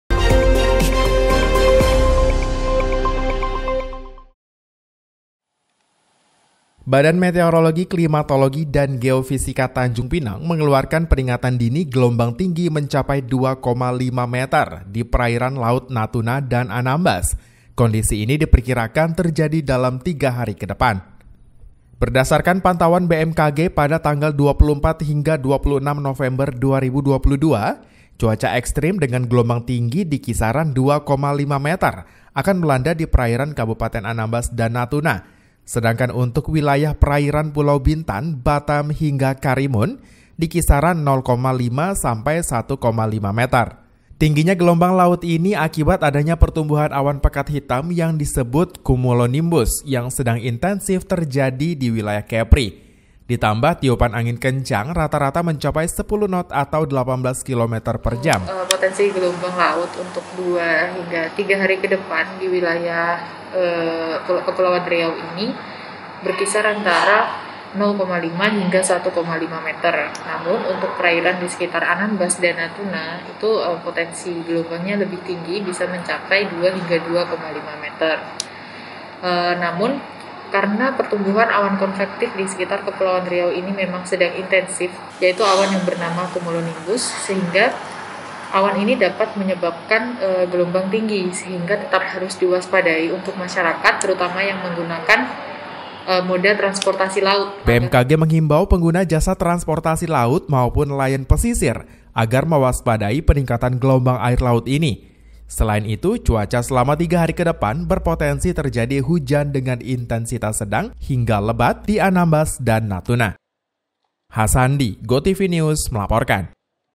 BMKG Imbau Waspadai Gelombang Tinggi di Perairan Anambas dan Natuna
GOTVNEWS, Tanjungpinang - Badan Meteorologi Klimatologi dan Geofisika Tanjungpinang mengeluarkan peringatan dini gelombang tinggi mencapai 2,5 meter di Perairan Laut Natuna dan Anambas. Kondisi ini diperkirakan terjadi dalam 3 hari kedepan.